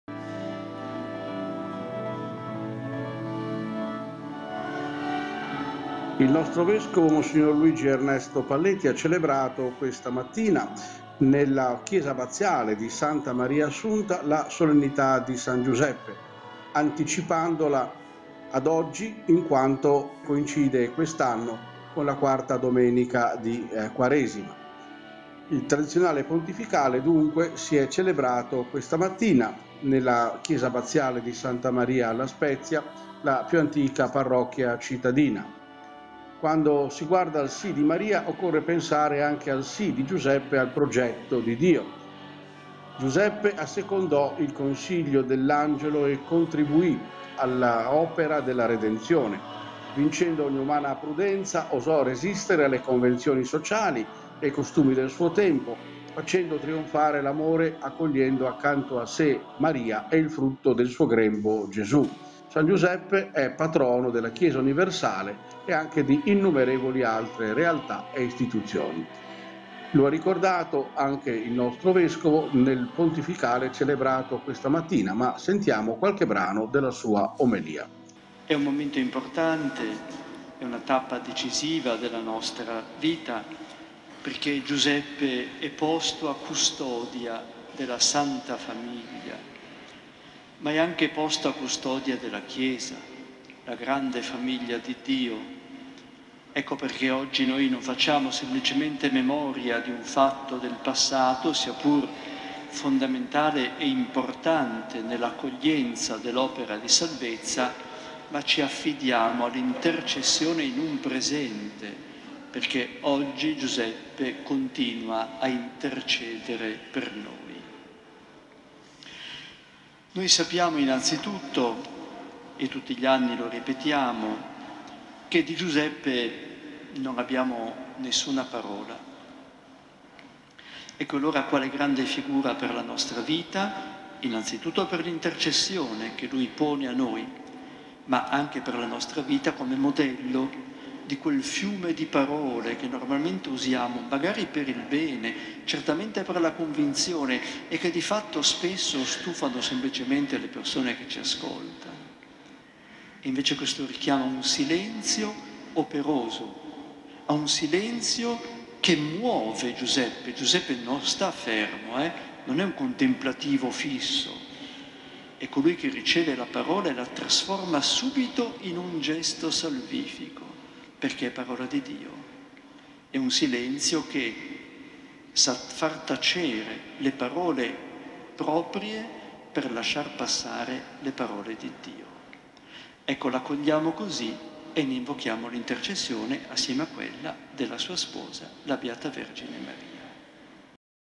San Giuseppe, la Messa oggi in Santa Maria 18-03-2023
Si è tenuta questa mattina nella Chiesa di Santa Maria in Piazza Beverini, la Messa in onore di San Giuseppe. A celebrare la funzione il Vescovo Diocesano Mons. Luigi Ernesto Palletti.